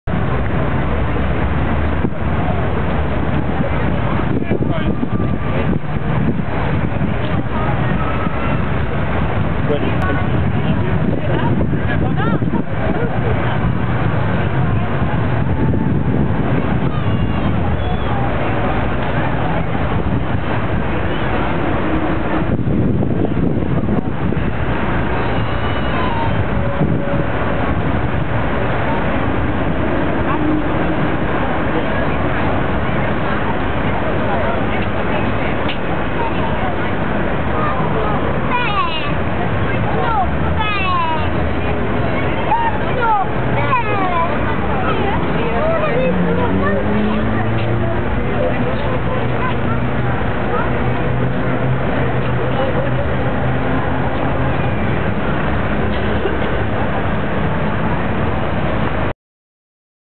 Giving Nelson a scrubbing 9th April 2008
It was 9th April 1838 that the National Gallery oppened which is opposite to Nelson's Column where men were giving him a good scrubbing today. One little kid got real grumpy because he couldnt climb on the lions.